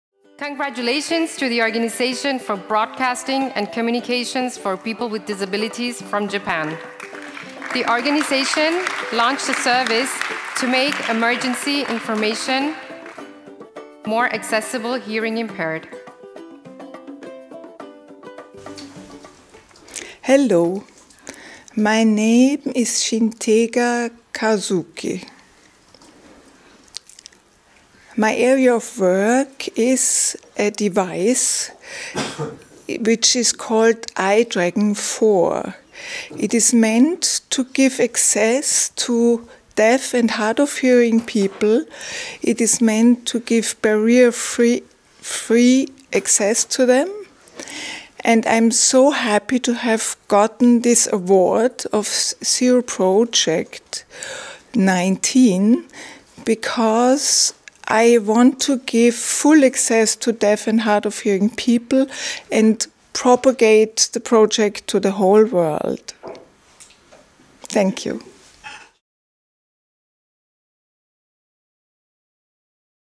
Organization Broadcasting Communications: A dedicated TV channel for the hearing impaired
Welcome to the awardee video of  Organization Broadcasting Communications´s Listen with Your Eyes, a winner of the 2019 Zero Project Award on Independent Living and Political Participation.

In 1998 the Organization for Broadcasting and Communications for People with Disabilities, a Japanese certified non-profit organization, launched a service to make emergency information more accessible to the hearing impaired. In 2017 they revised it to meet international standards. Internet Protocol Television set-top boxes add closed captioning and signing to TV content in real-time. The service is provided through a dedicated channel called “Listening With Your Eyes,” and based on its success the Japanese government is looking to expand the programme.

Speaker:
Shigeta Kazuki, Organization Broadcasting Communications, Japan